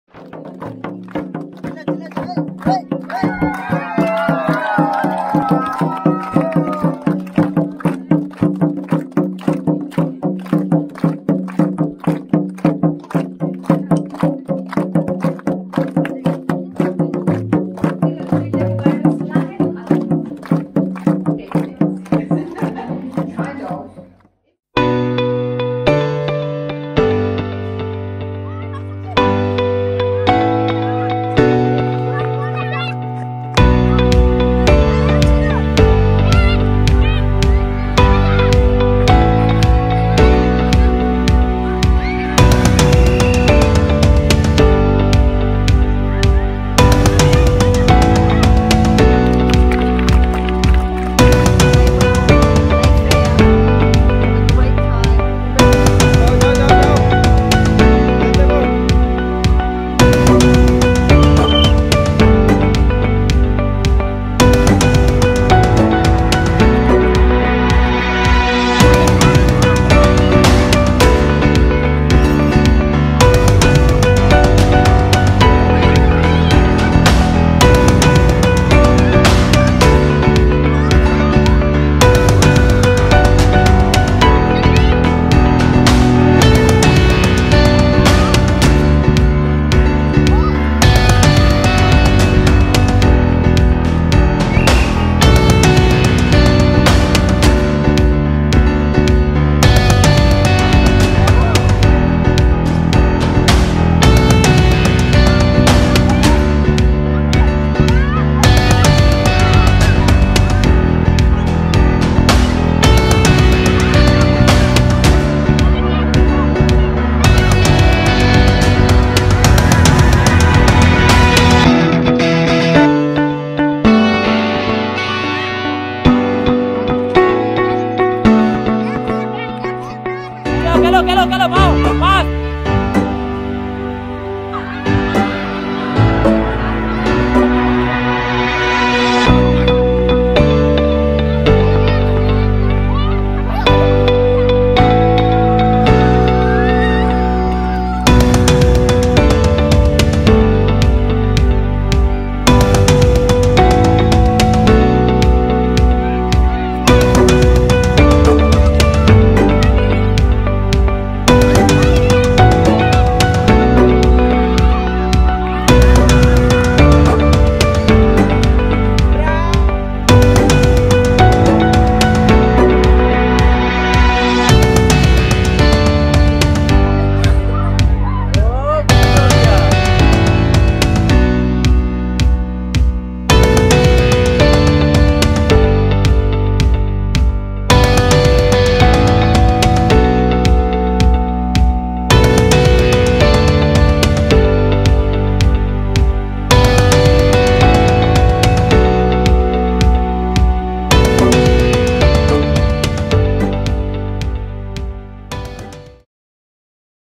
Let's Play Rugby Jharkhand - With Australian Consul-General!
In June 2022 Australian Consul General Rowan Ainsworth visited our Khelo Rugby project in Haripur Bazar in the Jarmundi Block of Jharkhand.
We were delighted to welcome Rowan and her colleagues to Jarmundi, the encouragement it gave to everyone was incredible.
The young people of Jarmundi are extraordinary, their resilience and spirit is outstanding.
Faced with the challenges of the pandemic and already difficult cicumstances to grow up in we are determined to keep supporting them in their mission to stay strong.

Keep supporting Khelo Rugby online: @KheloRugby